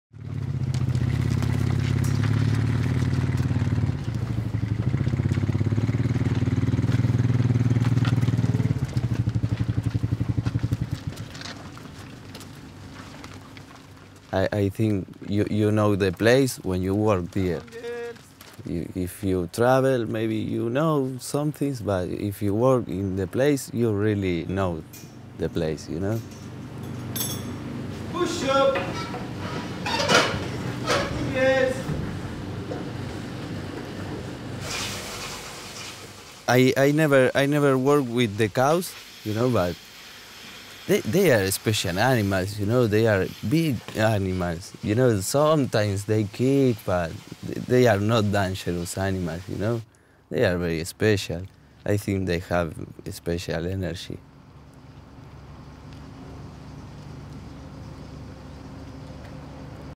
“Cows have a special energy” | MATIAS by Human Postcards
ARGENTINA/NEW ZEALAND | Matias was born and raised in Argentina's largest city, Buenos Aires. For the past 7 months he has worked as a Dairy Farmer in the small rural town of Whataroa. Before his life in New Zealand, Mathias spoke no English and had never touched a cow in his life. He shares with us his experience.